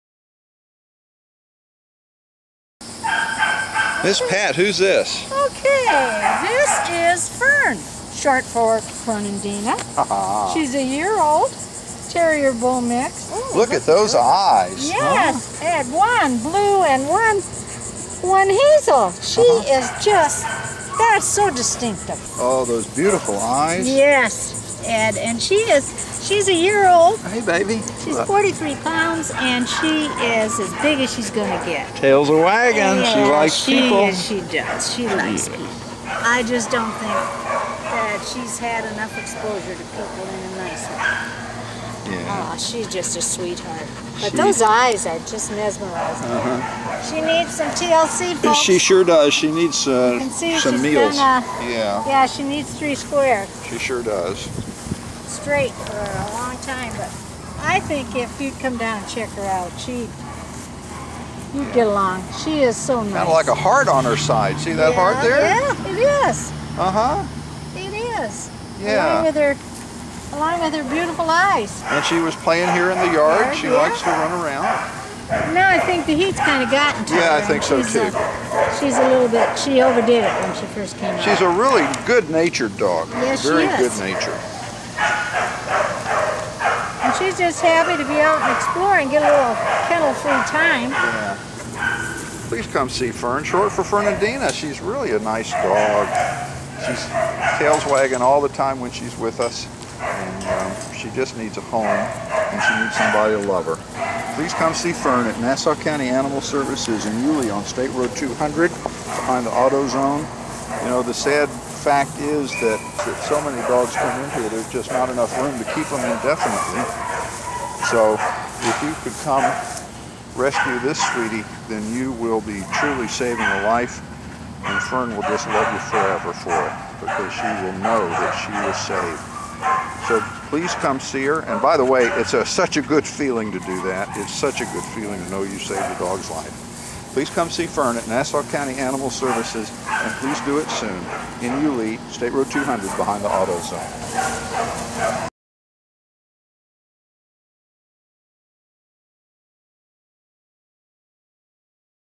Please rescue Fern!
Fern (short for Fernandina) is a good-natured, friendly and affectionate young terrier mix who needs a loving home. Please come see him at Nassau County Animal Services in Yulee, FL very soon!